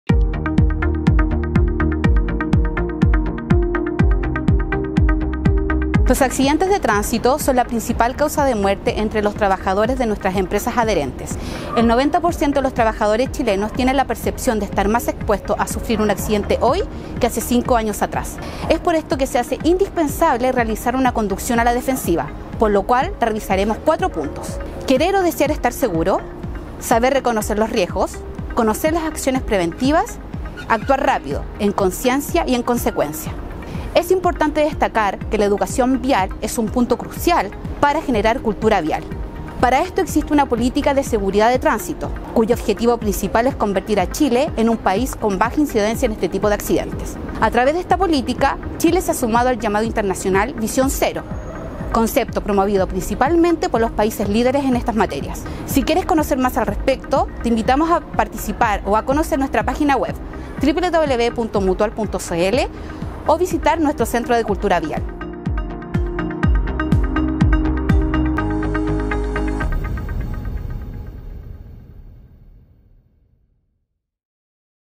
Cultura Vial, de Mutual de Seguridad
Revisa aquí esta didactica cápsula acerca de la importancia de la Seguridad Vial y la prevención de accidentes de tránsito, consejos entregados por Susana Barrera, Experta en Prevención de Accidentes de Mutual de Seguridad.